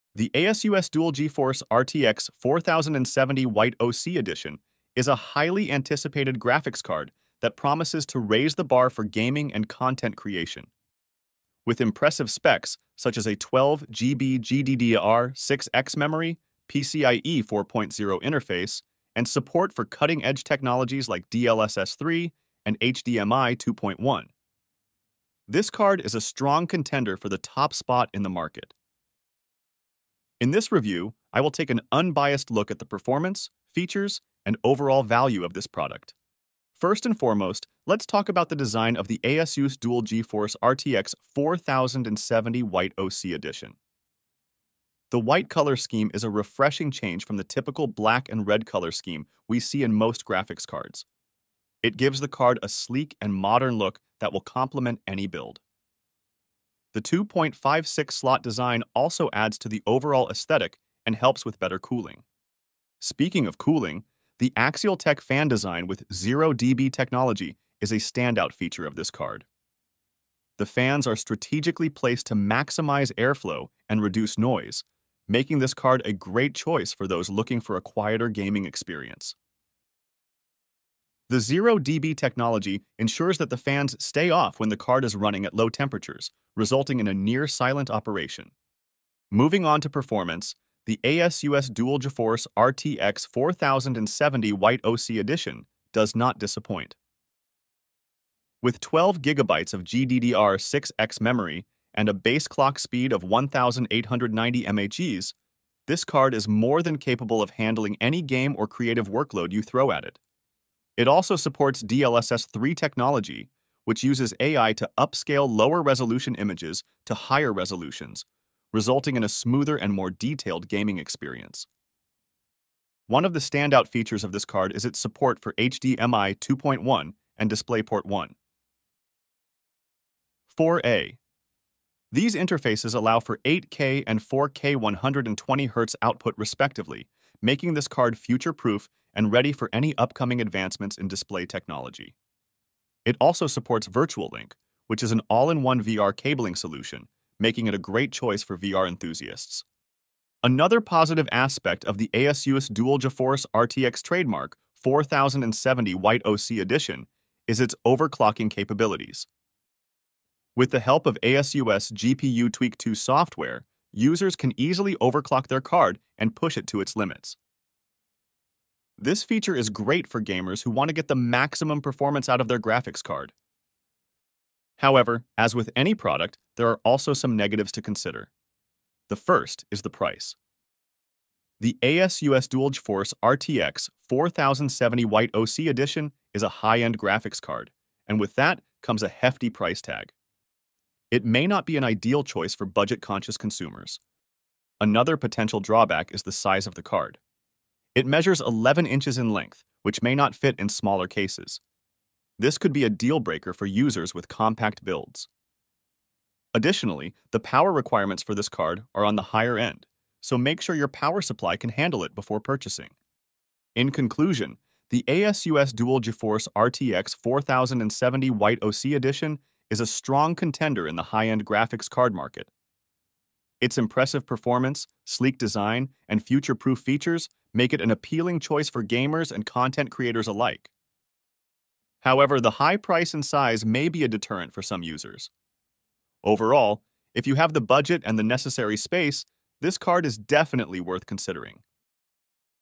ASUS Dual GeForce RTX 4070 - Worth Buying?
ASUS Dual GeForce RTX 4070

Discover the power and style of the ASUS Dual GeForce RTX™ 4070 White OC Edition graphics card. Featuring PCIe 4.0, 12GB of GDDR6X memory, DLSS 3 technology, HDMI 2.1, DisplayPort 1.4a, and a 2.56-slot design, this card delivers stunning performance for gaming and content creation. The Axial-tech fan design ensures efficient cooling, while 0dB Technology keeps things quiet during light loads. Elevate your gaming experience with the ASUS Dual GeForce RTX™ 4070 White OC Edition.